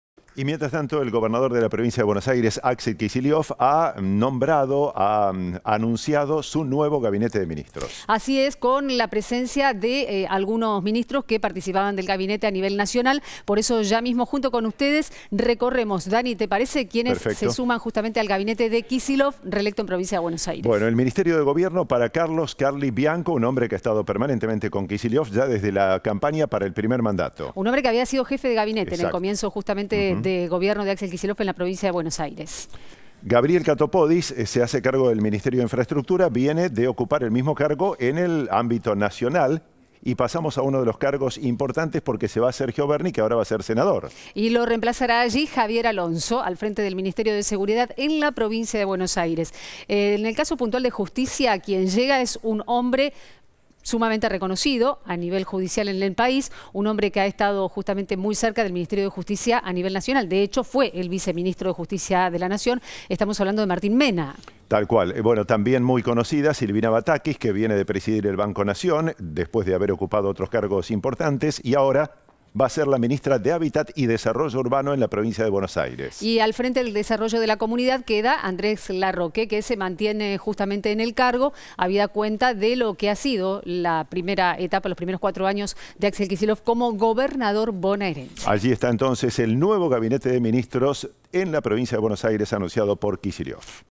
Continuidad y cambios en el nuevo gabinete de Axel Kicillof
La gobernación bonaerense anunció hoy los nombres del equipo que acompañará al economista en su segunda gestión a cargo de la provincia más grande del  país. Quiénes se van, quiénes se quedan y qué figuras desembarcan en el gabinete del peronista.